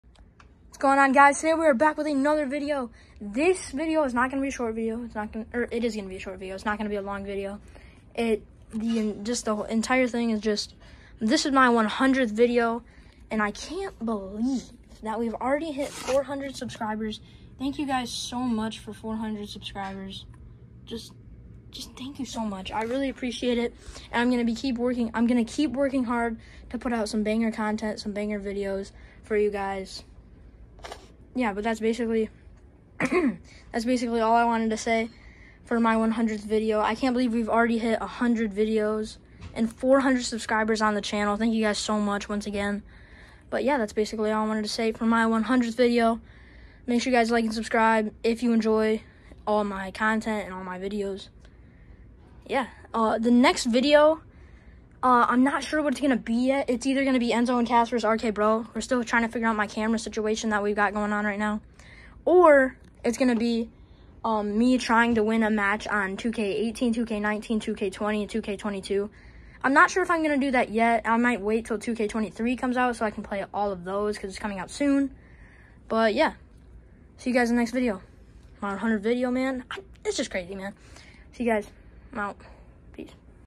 My 100th Video
This my 100th video￼ on the channel in the video I Talk about the next video on the channel we also talk about this video and about 400 subscribers￼.￼